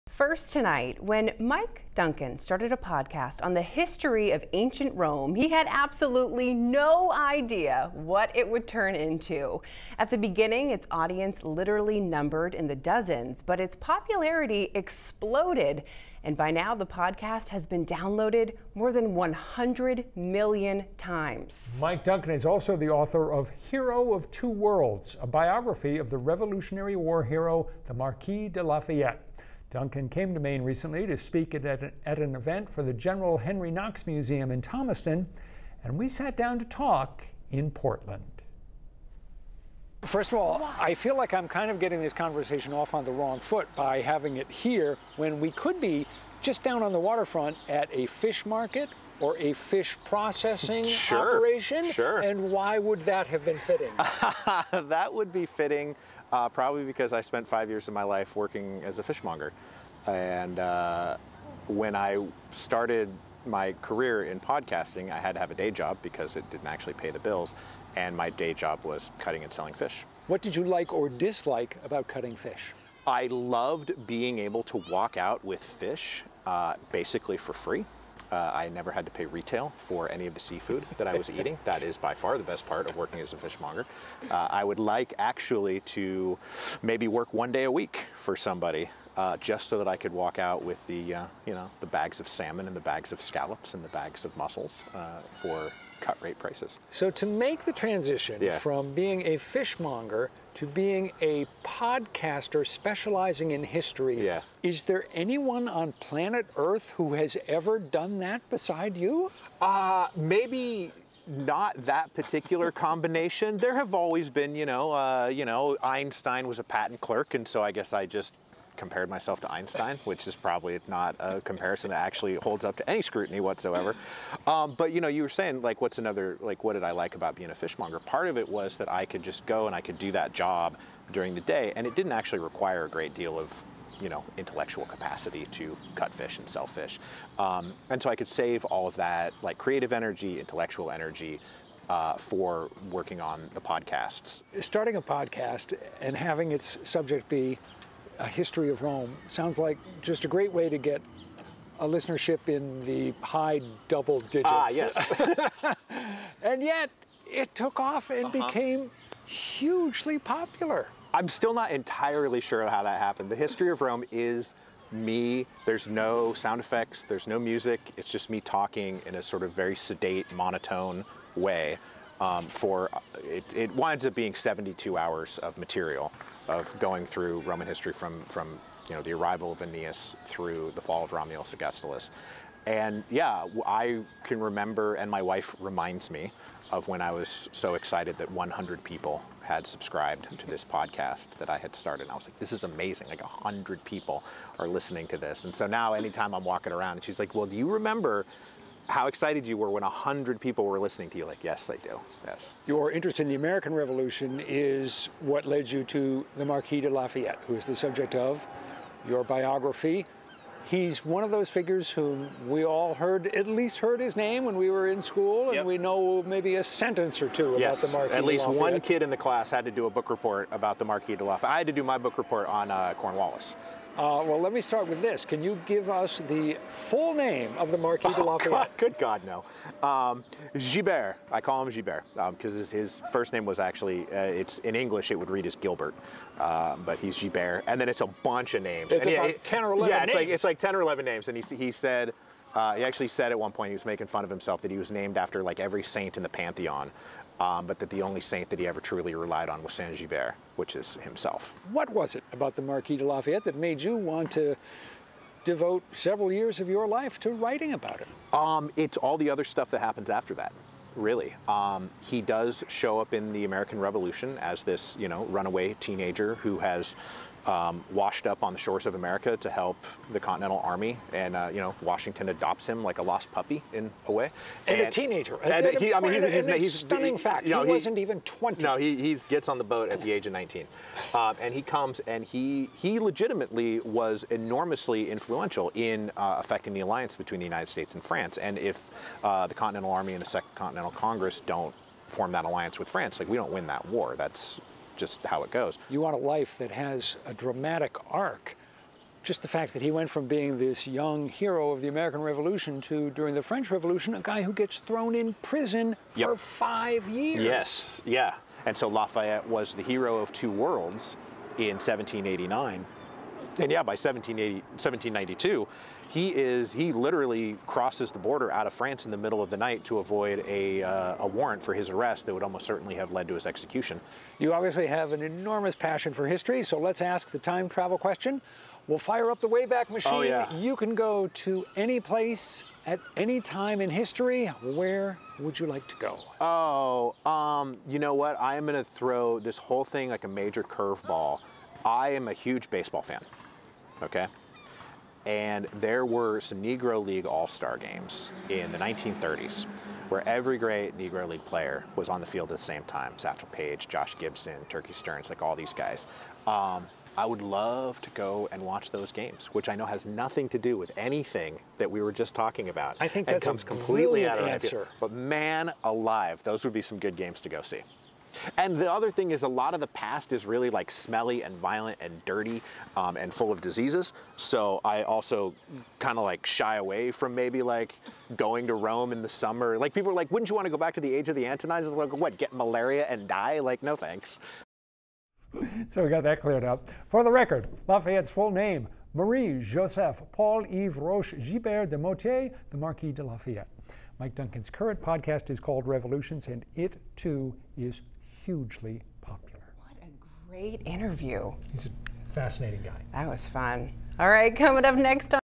The weird career path that took Mike Duncan from a fish market to an immensely popular podcast
Although he no longer gets free seafood, he’s made up for it with an audience of hundreds of thousands